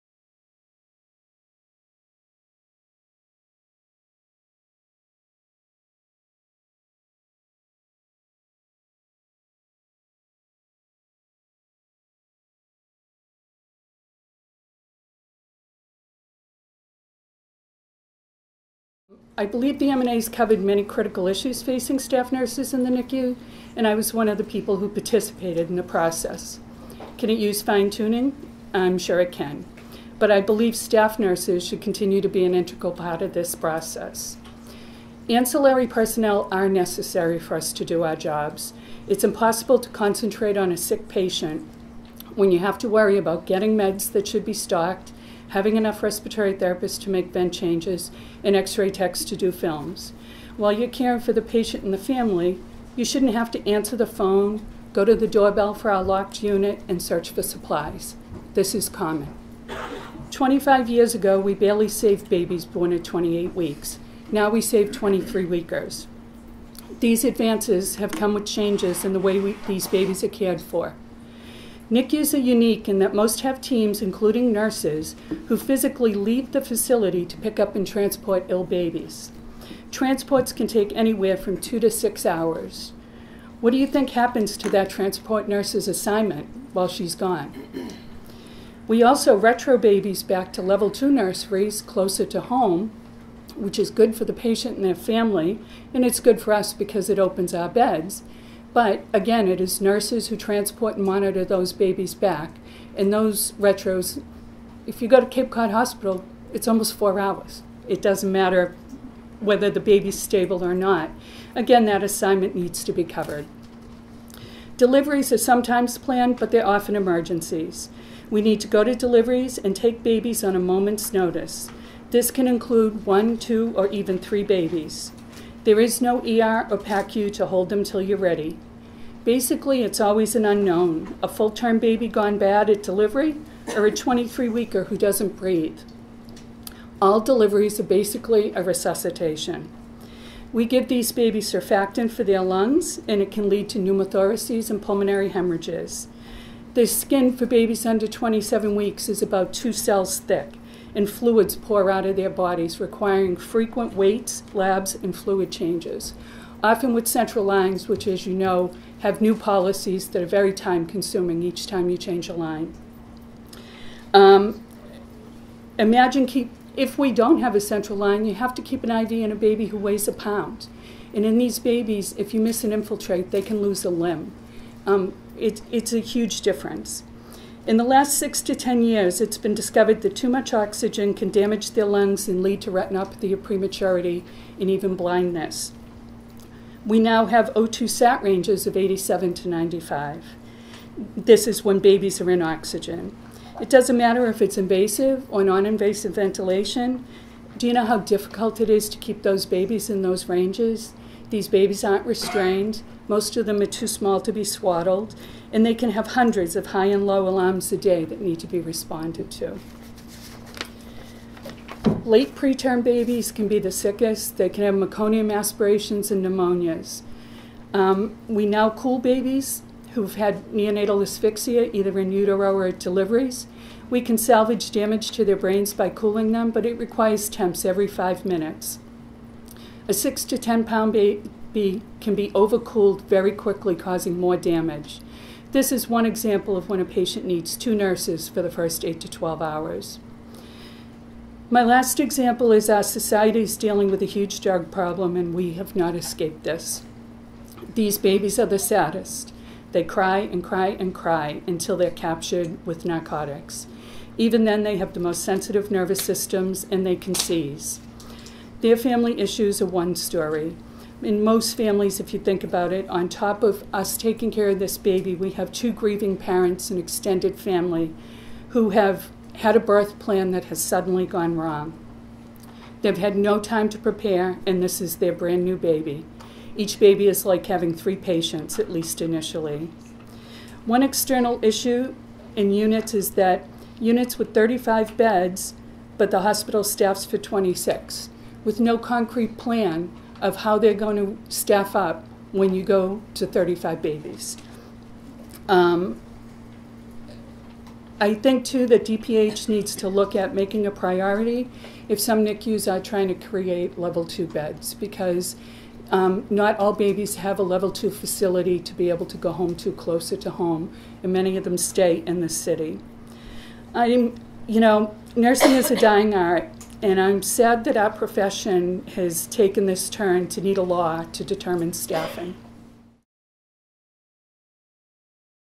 Maureen Keeley, RN testifies at Health Commission Listening Session
On September 28, the state implemented a new law that requires hospitals to adhere to safe patient limits for registered nurses who work in all the state's hospital intensive care units, ensuring that no nurse can be assigned more than one patient or in certain circumstances, no more than two patients based on the assessment of the staff nurses (not managers or supervisors) on that unit.

The new law, An Act for Patient Limits in All Hospital Intensive Care Units, applies to ALL types of intensive care units. The law calls for the Health Policy Commission to establish criteria for an acuity tool to be used by all hospitals to assist staff nurses in determining if their patient is stable enough to allow for a second patient to be added to the nurses' assignment.

As part of that process, the Health Policy Commission (HPC) will conduct a Listening Session on the implementation of the new law specifically focused on helping the Commission formulate the "acuity tool," the method of public reporting of staffing compliance, and identifying three to five patient safety quality indicators to be measured and reported by hospitals.

The MNA/NNU will be testifying at this listening session and we have a number of ICU nurses providing testimony from hospitals throughout the state